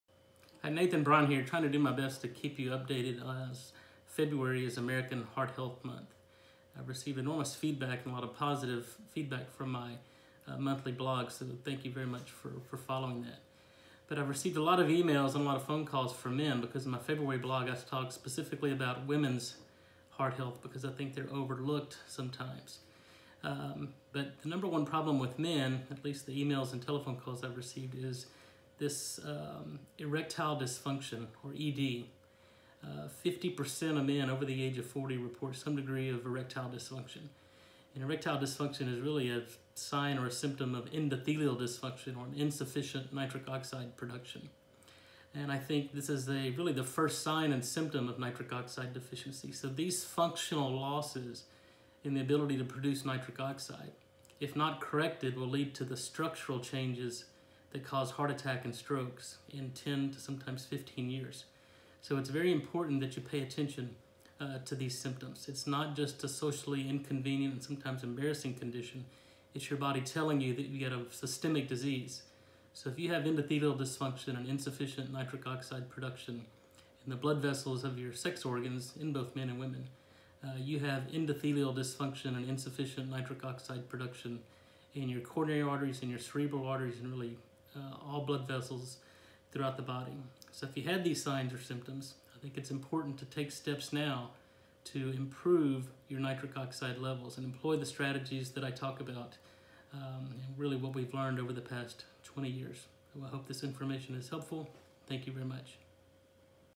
Nitric oxide and Erectile Dysfunction video Feb 2019
Nitric oxide is a powerful signaling molecule that plays a key role in regulating blood flow, particularly to the penis. When nitric oxide levels are low, it can lead to impaired blood flow to the genital area, contributing to erectile dysfunction.